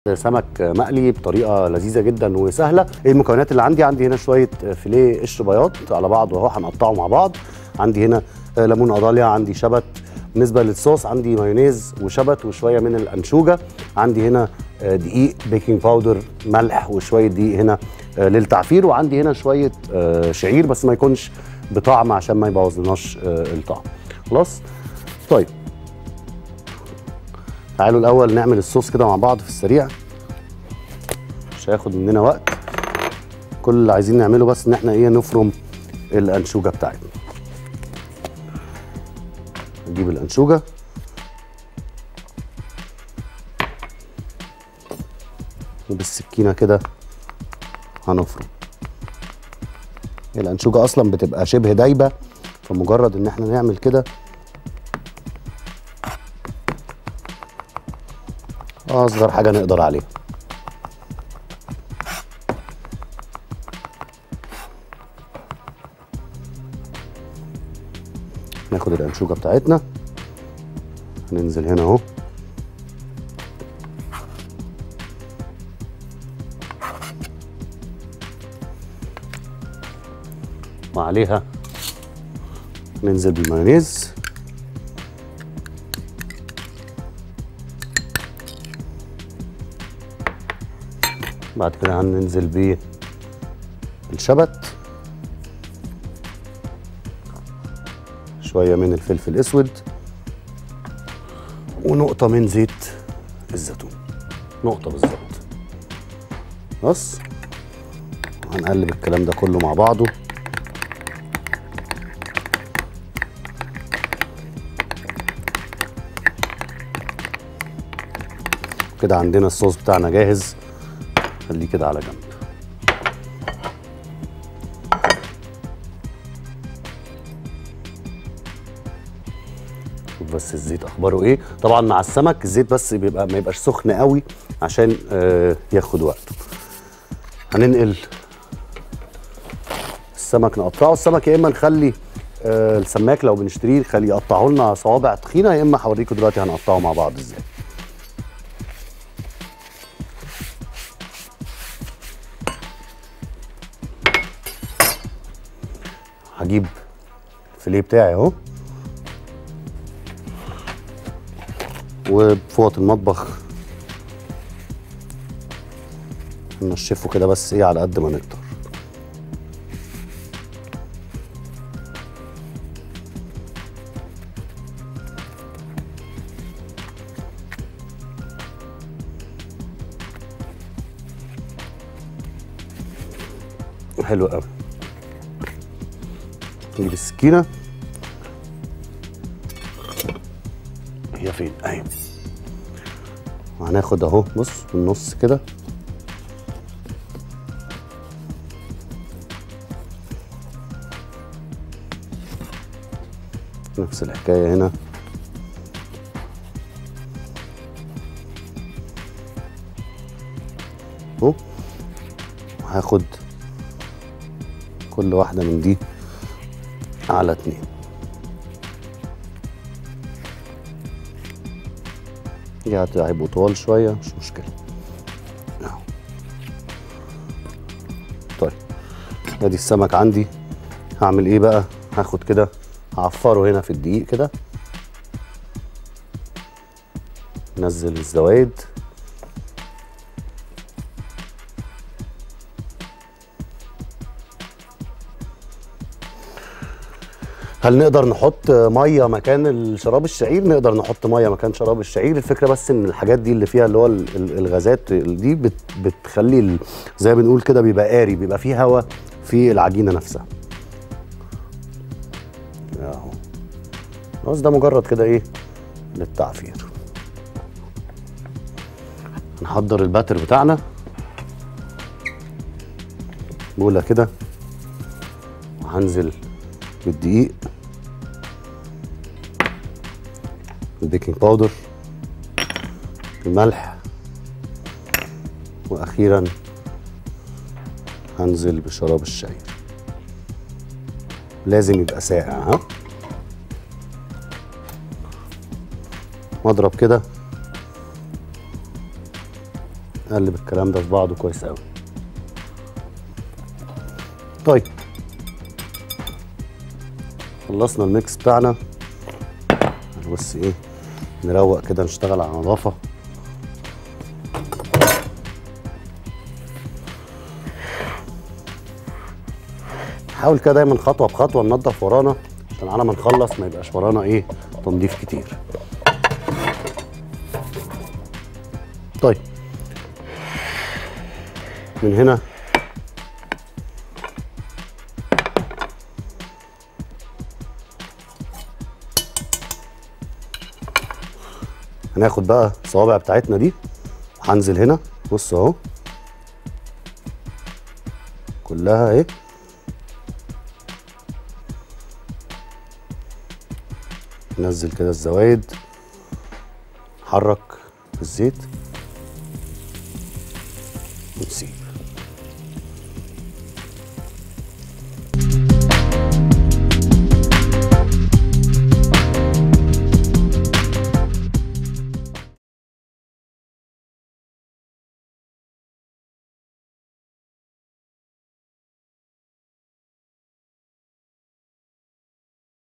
فيش ستيكس | عمرو إسماعيل
طريقة عمل فيش ستيكس  مع الشيف عمر اسماعيل في  نأنأة